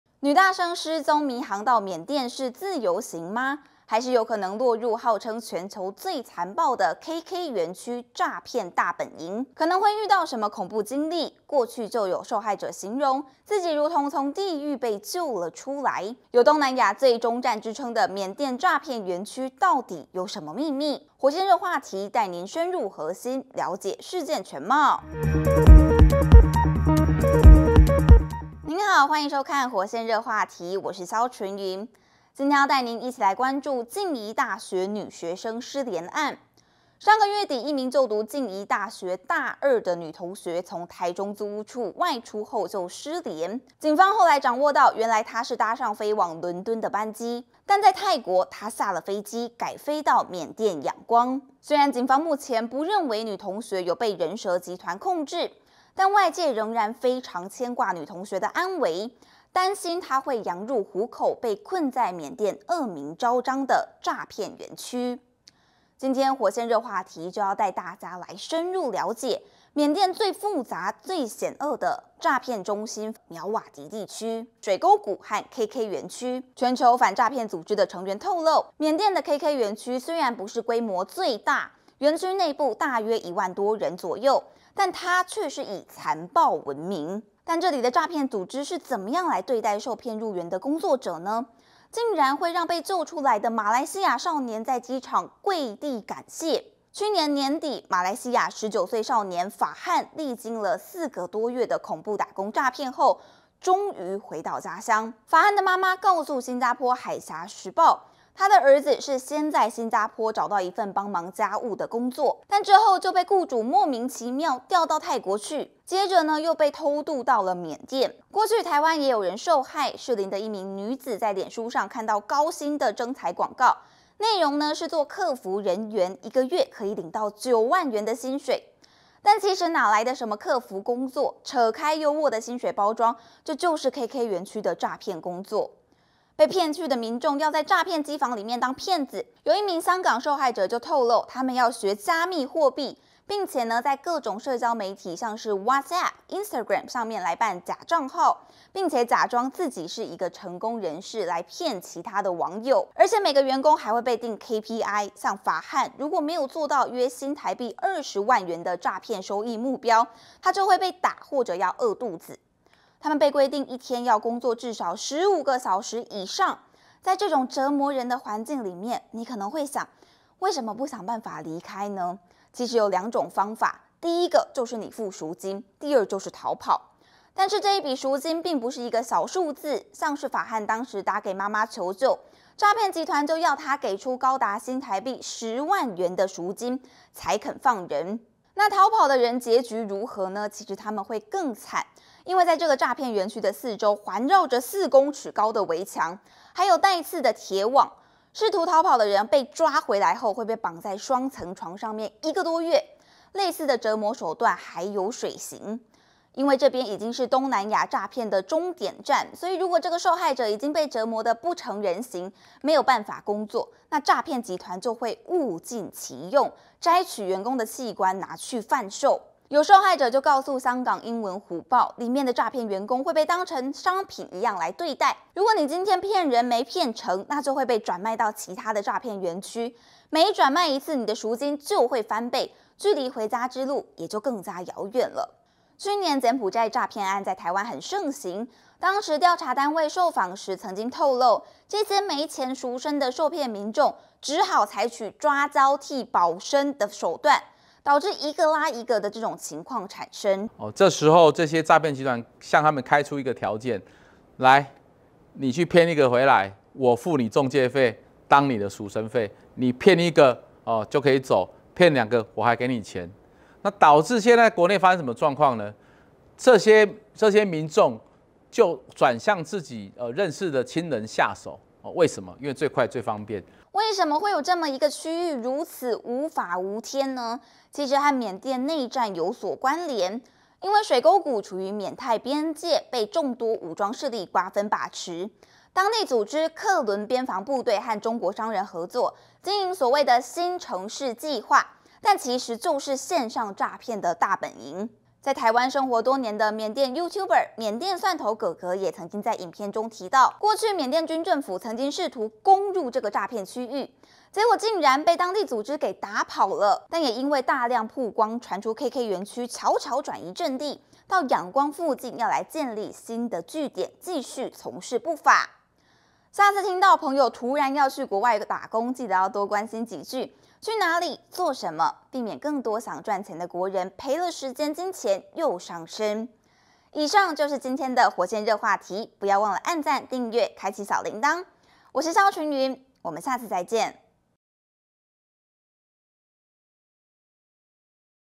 火線熱話題》緬甸詐騙煉獄大揭秘！ＫＫ園區「活摘器官」內幕曝光「這兩招」讓受害者生不如死！緬甸軍隊也攻不進去？
【看更多報導】

獨家》靜宜女大生緬甸失蹤案 疑與涉詐新男友有關

失聯靜宜女大生入境泰國後手機曾開通 「前男友」有詐欺前科

女學生失蹤9天疑入境泰國 靜宜大學稱已動員協尋、提醒師生留意